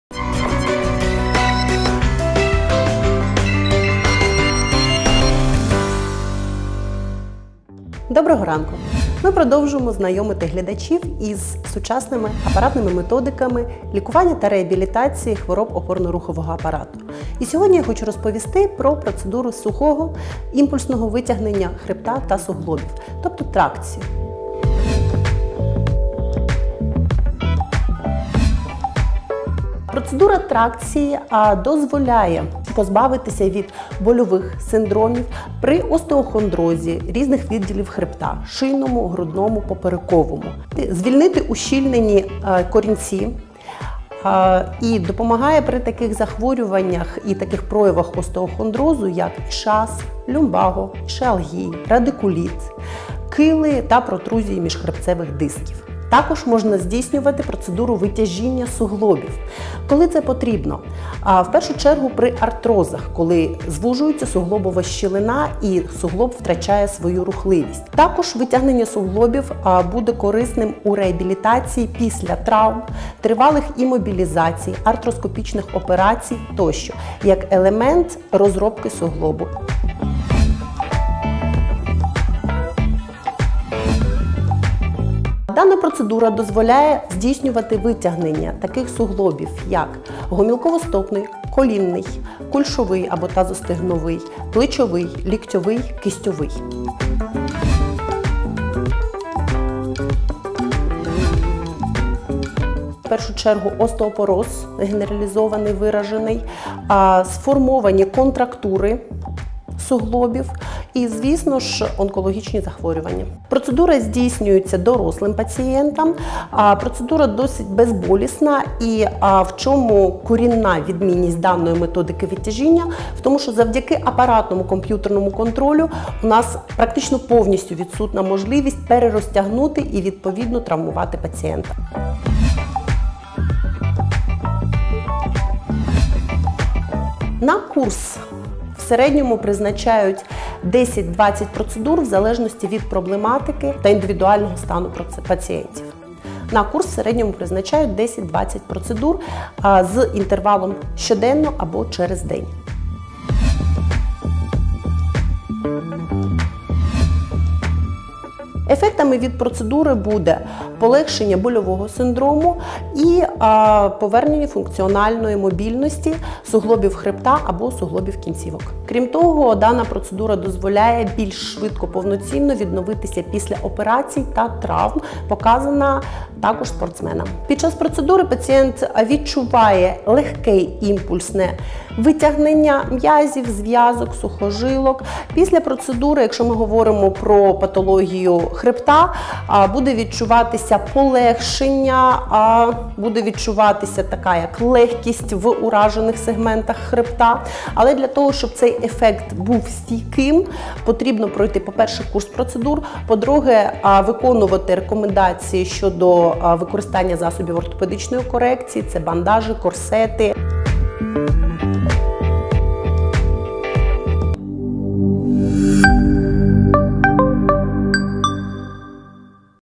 31.05.2019. Новий день. Тракція - процедура сухого імпульсного витягування хребта та суглобів
Витягування хребта та суглобів дозволяє позбутися болю різного походження. Цей  метод рекомендують при лікуванні остеохондрозу, артрозу, радикуліті,при травмах.
Тривалість всієї процедури - не більше однієї години. Під час тракції пацієнт може відчувати дискомфорт, але больові відчуття при цьому неприйнятні. У більшості випадків сухе тракційне витягування проводиться курсом, що складається з декількох процедур.
Подробиці про цей метод далі у сюжеті.